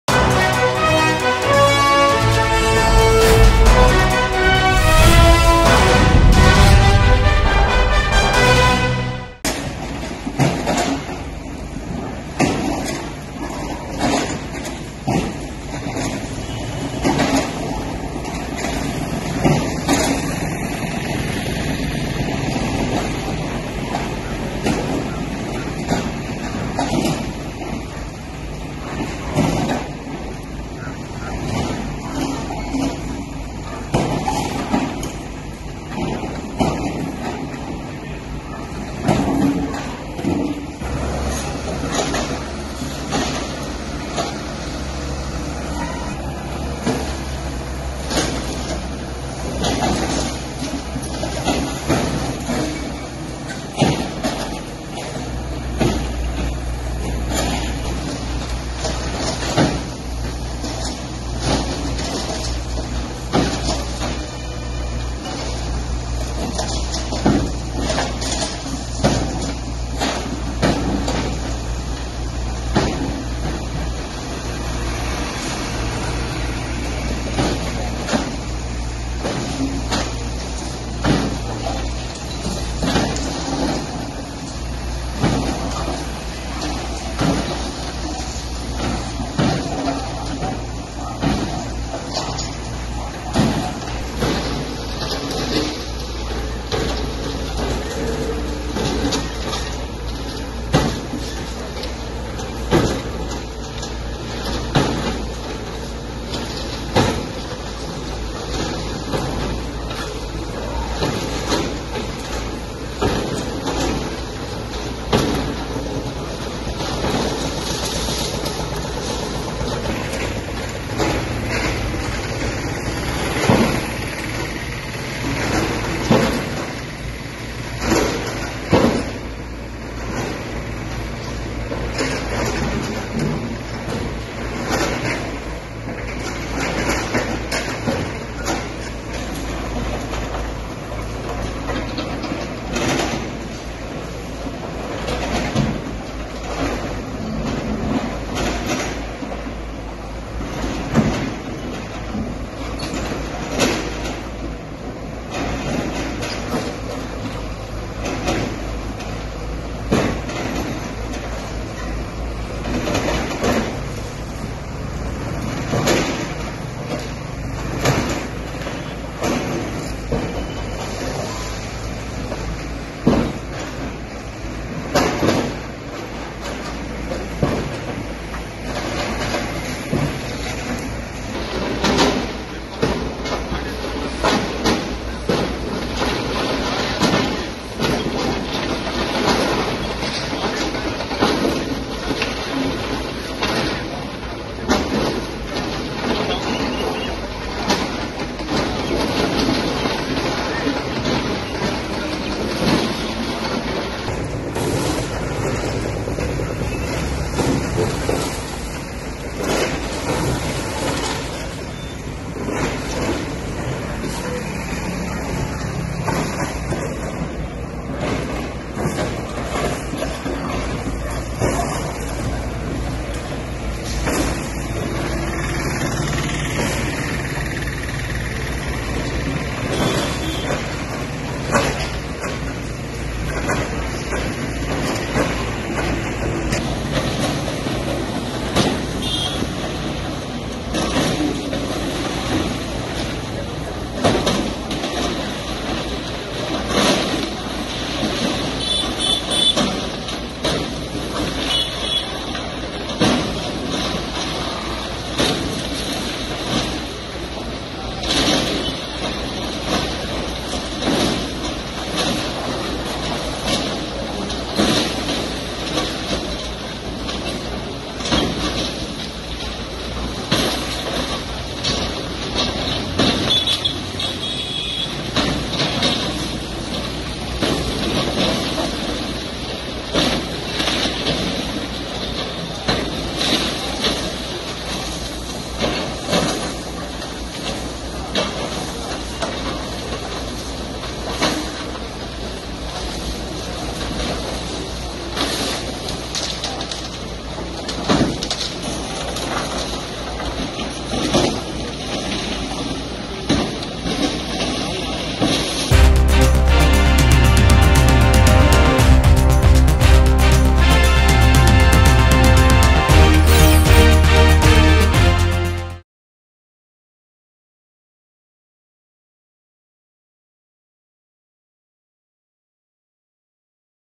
YCP Office Demolition At Tadepalli | CRDA | YSRCP | AP Political News | YS Jagan | Mango News
YCP Office Demolition At Tadepalli. CRDA officials have initiated an operation to demolish an under-construction party office belonging to the YSRCP in Tadepalli, Guntur district.